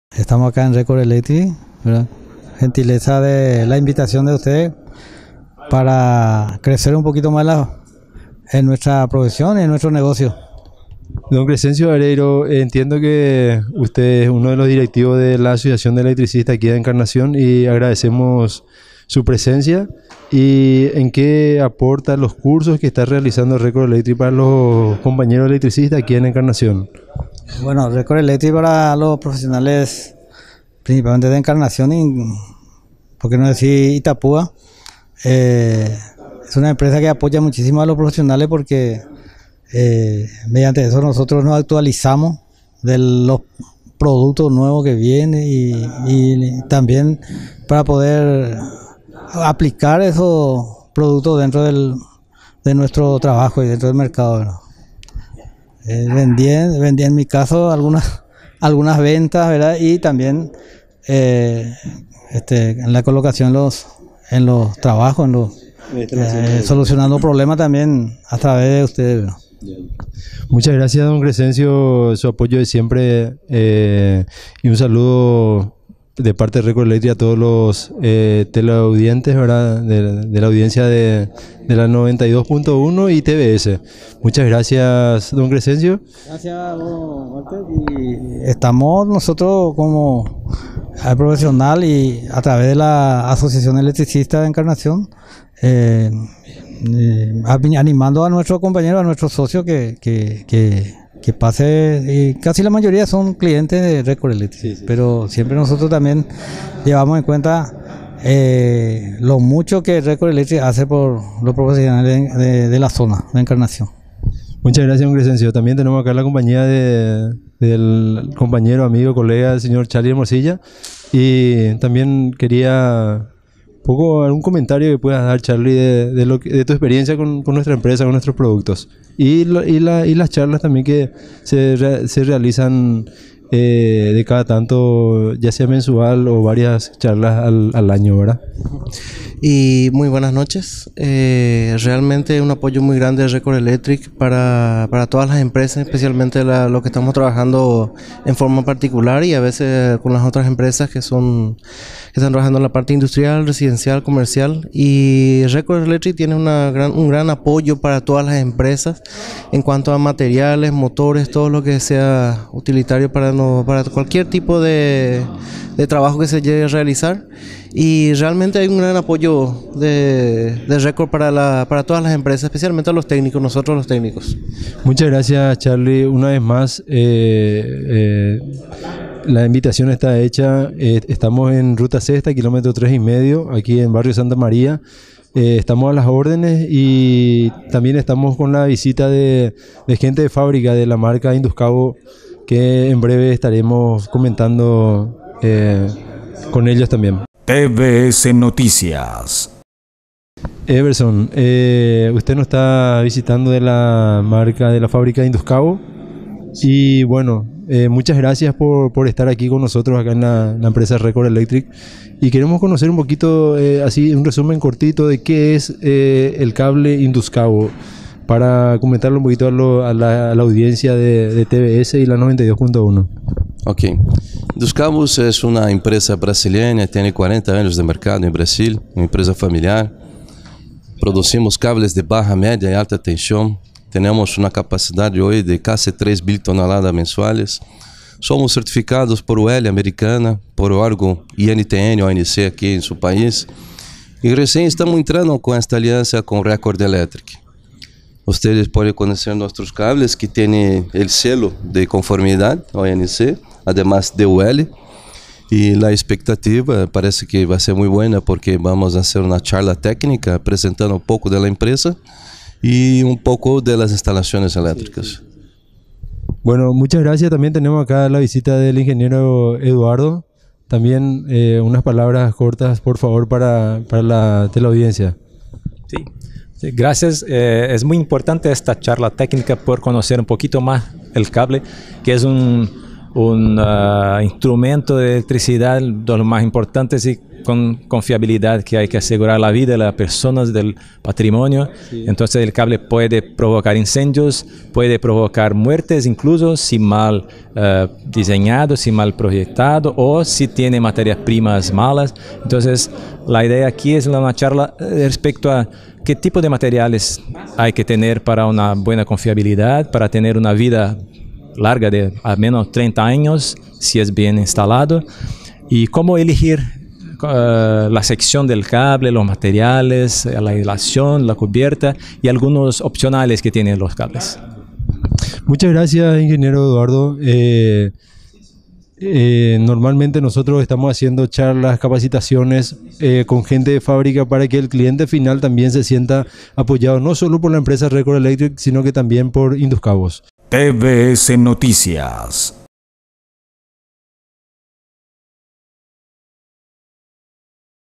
RECORD ELECTRIC
La empresa Record Electric junto a la empresa Induscabos, realizaron una charla de capacitación a los profesionales electricistas.